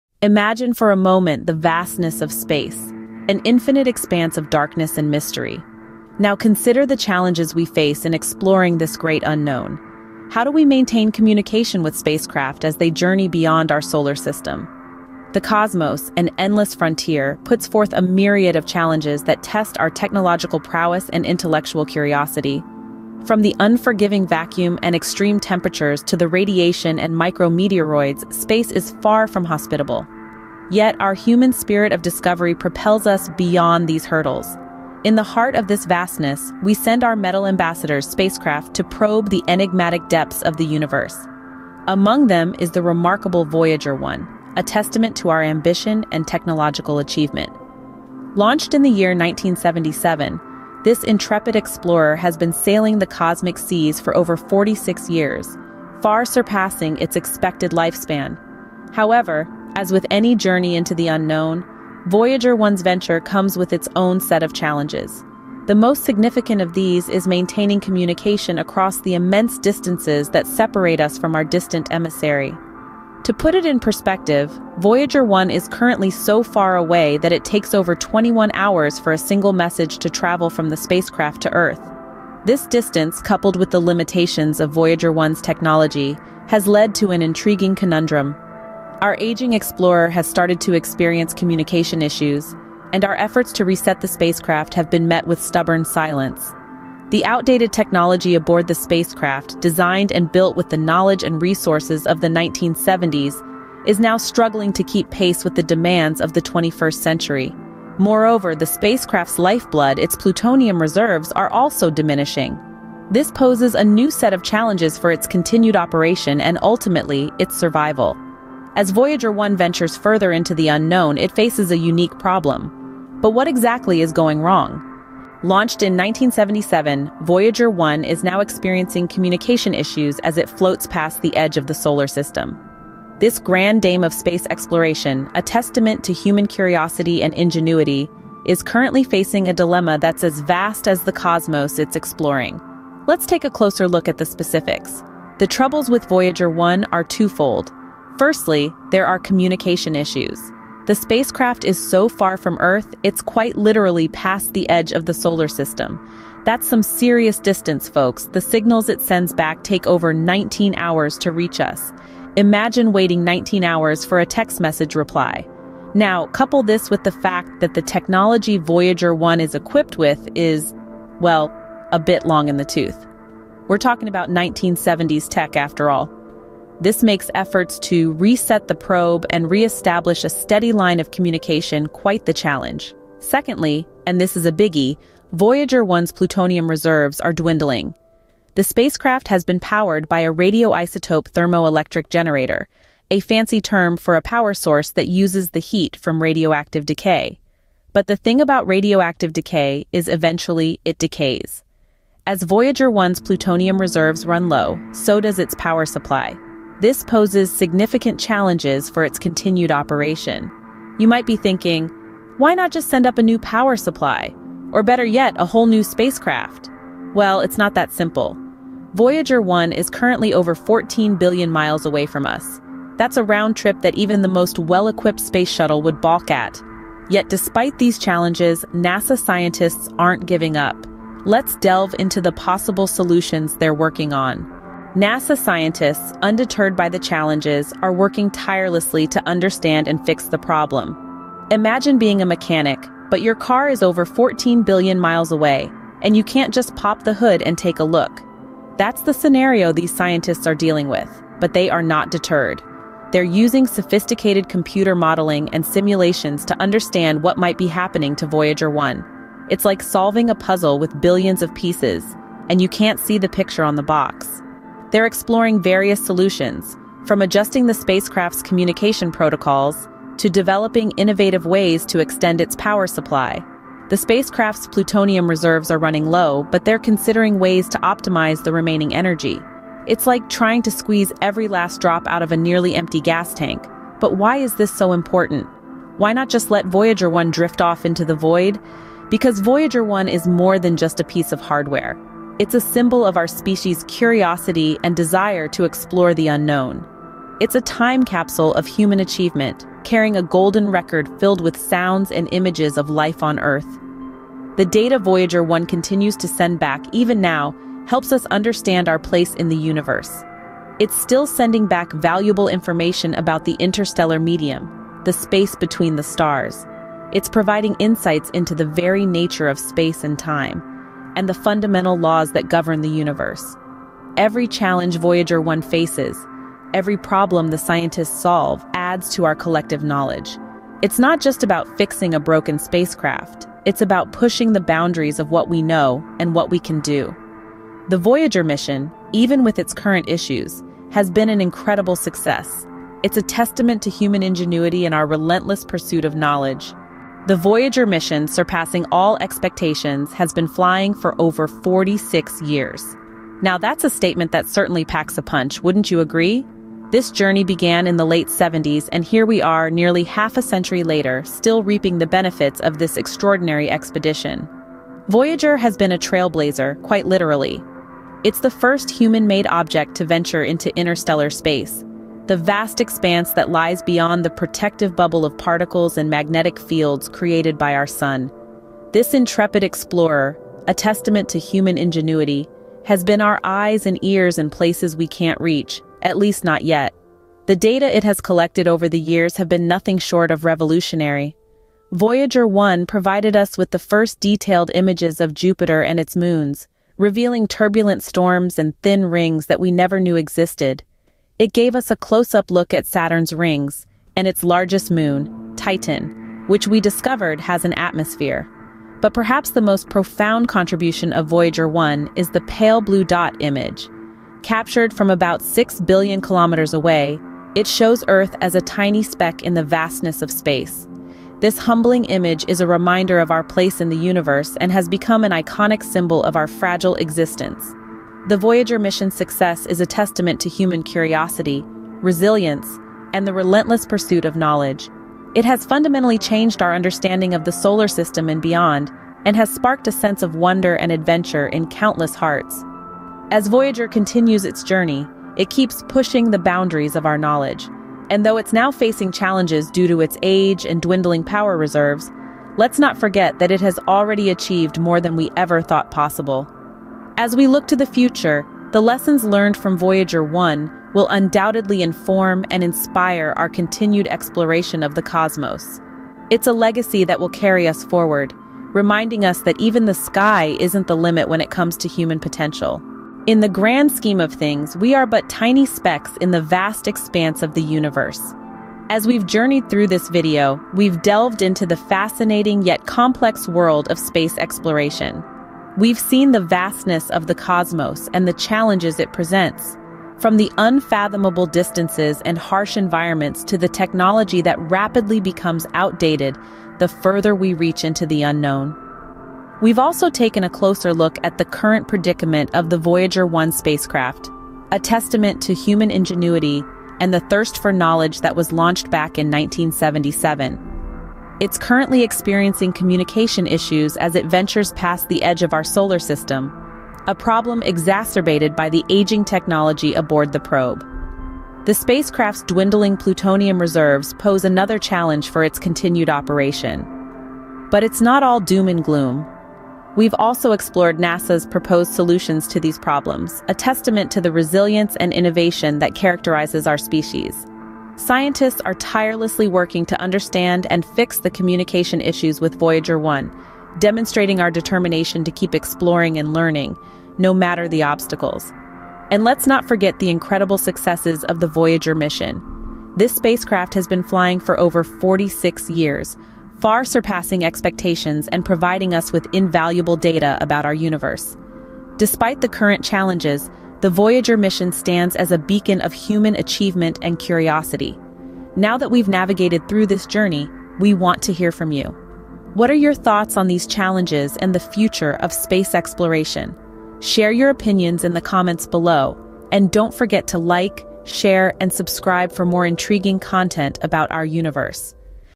🚀Breaking News: NASA's Voyager 1 Is Acting Up After 45 Years🌎
Dive into the chilling mystery of Voyager 1's latest transmissions with our latest video! 🚀💻 What could the strange, incomprehensible signals from deep space mean? Are we on the verge of a groundbreaking discovery, or is it a glitch from the cosmos? Join us as we explore the baffling enigma surrounding NASA's most distant emissary.